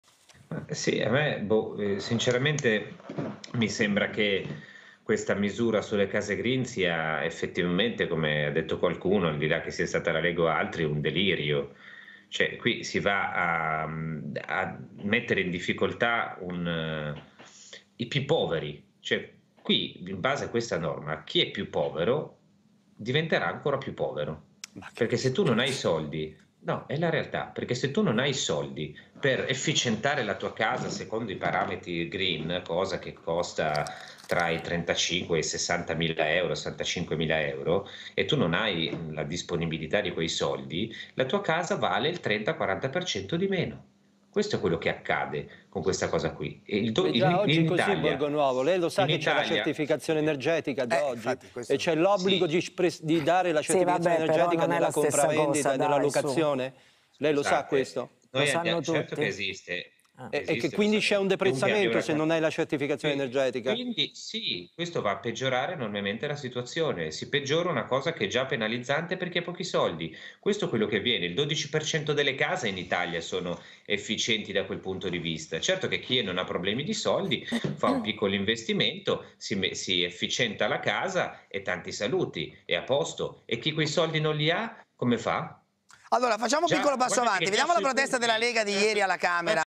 Case green, Francesco Borgonovo: "Un delirio, si va a mettere in difficoltà i più poveri che ...
Francesco Borgonovo (La Verità): "Si va a mettere in difficoltà i più poveri. Se tu non hai i soldi per efficientare la tua casa secondo i parametri green, cosa che costa tra i 35 e i 60mila euro, la tua casa vale il 30-40% in meno".